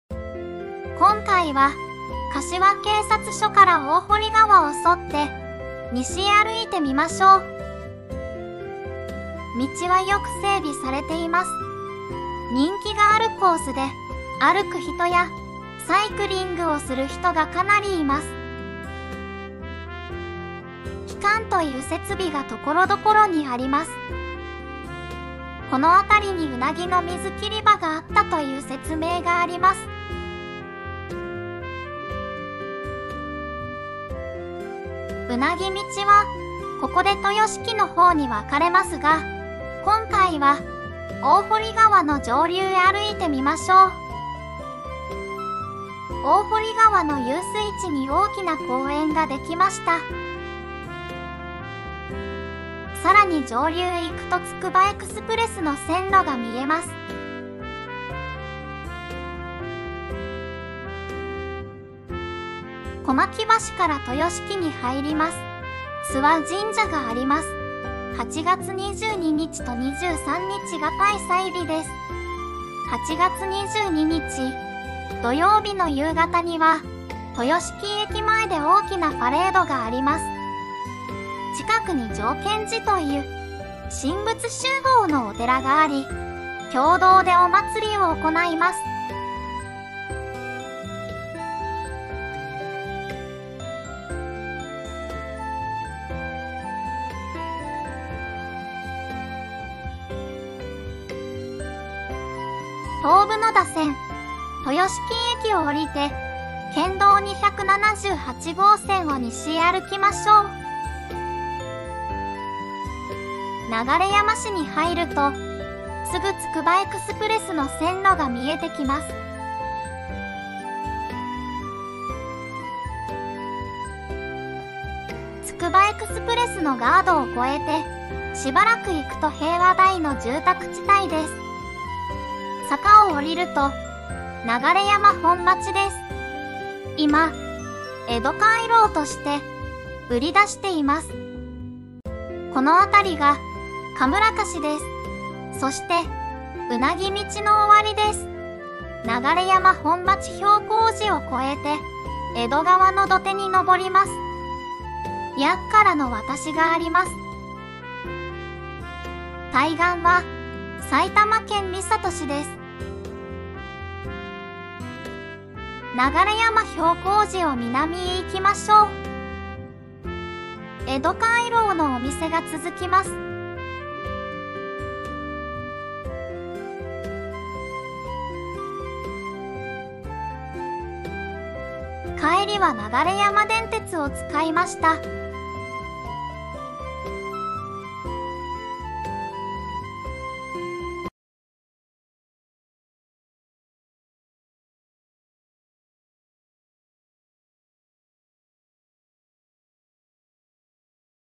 ウナギ道 その2
江戸時代から明治の中ごろまで　柏市と流山市のあいだに　うなぎ道がありました。利根川のうなぎを江戸に　はこびました。